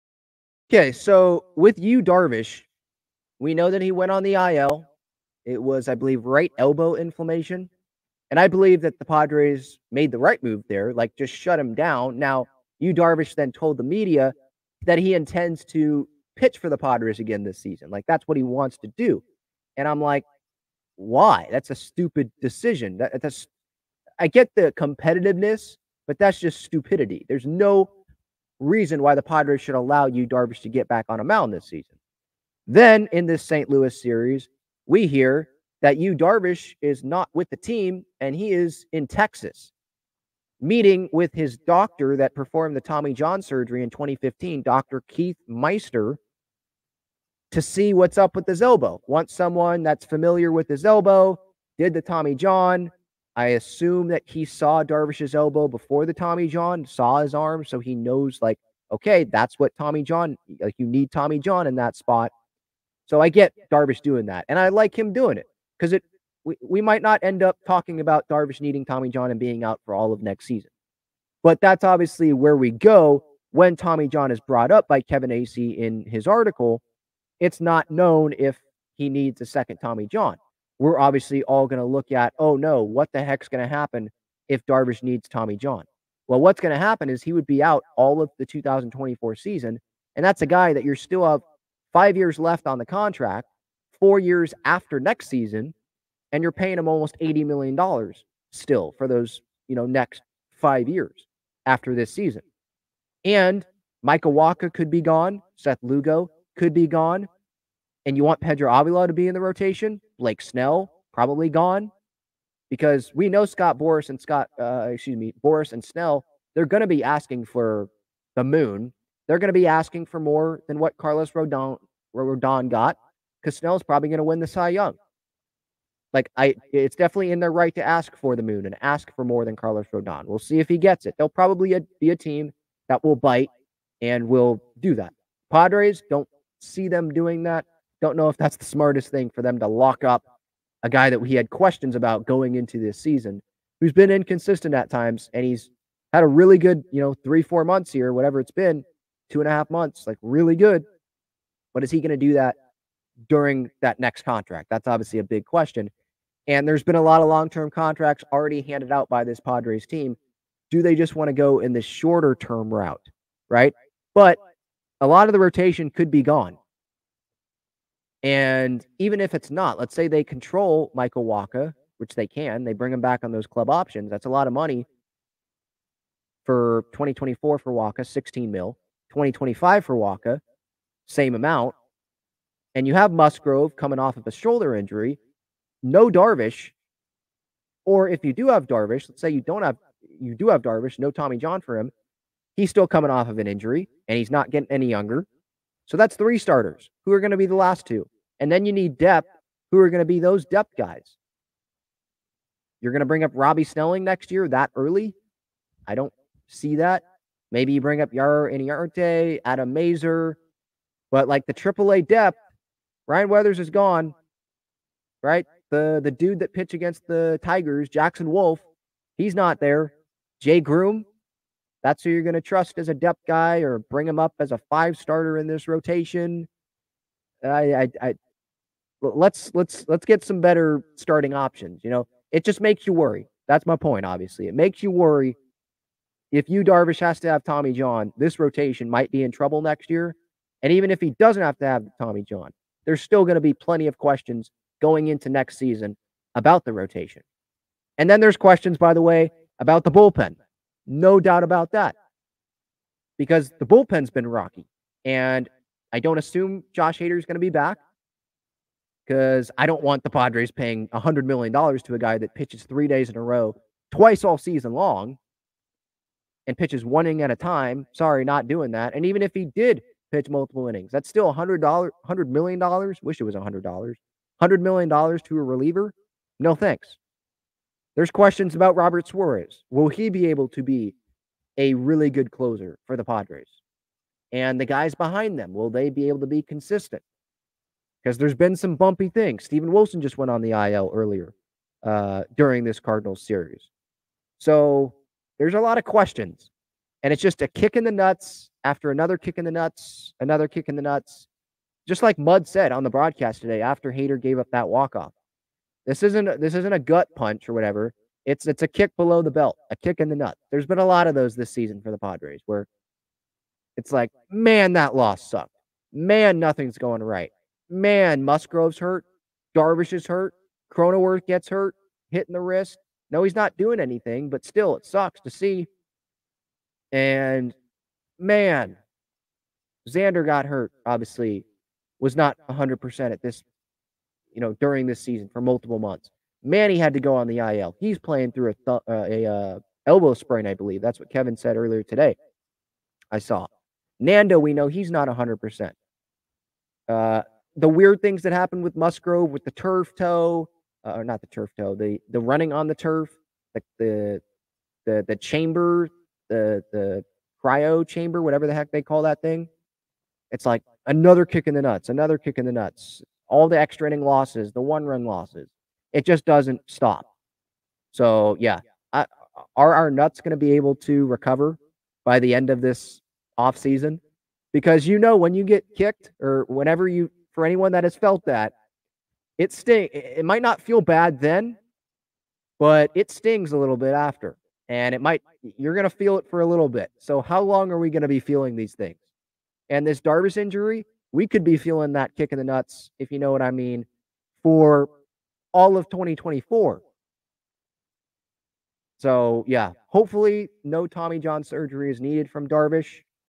More Padres question marks emerge if Yu Darvish needs Tommy John surgery
On Wednesday, Yu Darvish went to see the same doctor who performed his Tommy John surgery in 2015 to see if his arm's okay...

Check out some great Padres bobbleheads, footwear, bracelets and collectibles at FOCO!